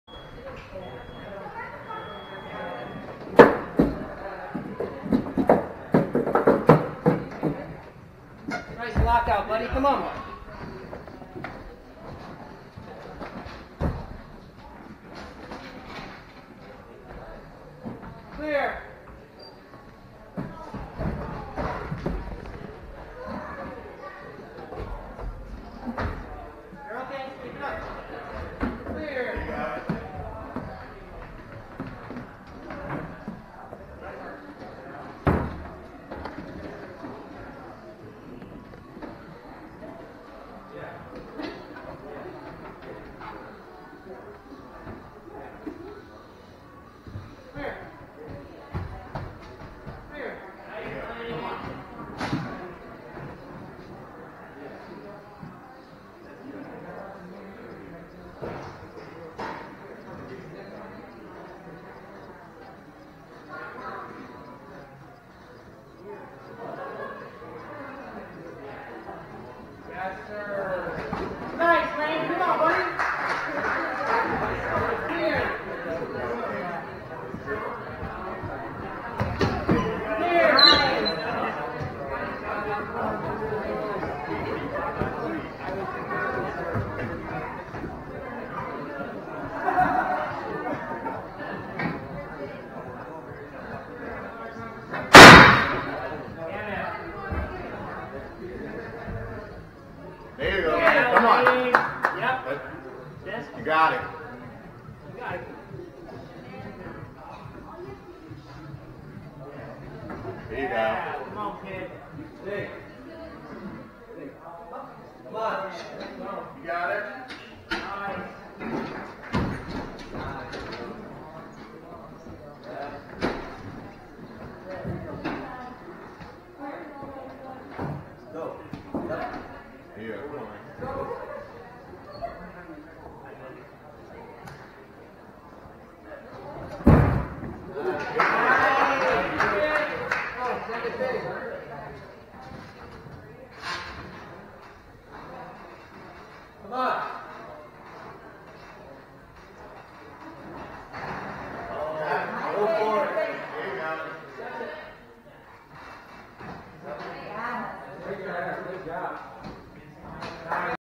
Lane Klein 2nd Place Build A Machine Fitness (#4) | National Ninja League Season 6
Lane Klein finishes the course in the second fastest time.